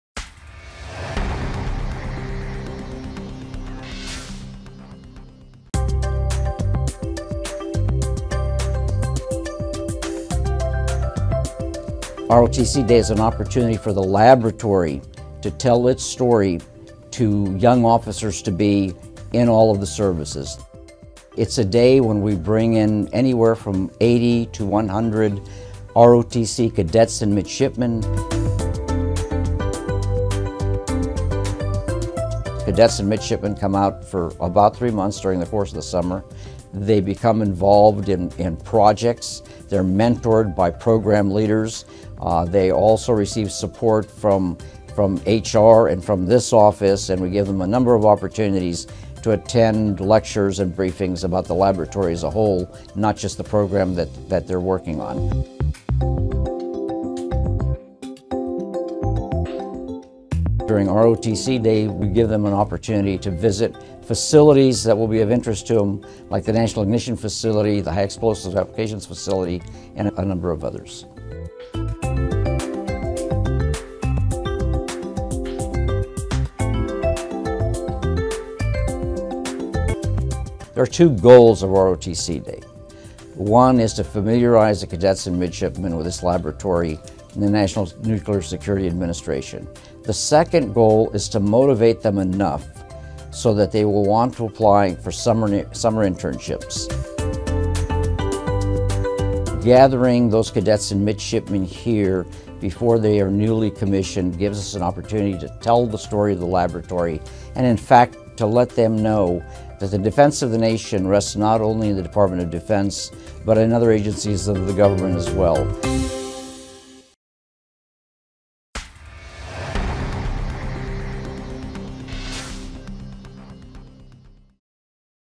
ROTC Day at the Lab
Lawrence Livermore National Laboratory's ROTC Day is an annual opportunity for ROTC cadets, midshipmen and cadre to learn about the programs at LLNL that support the Department of Defense, the military and national security, including internships.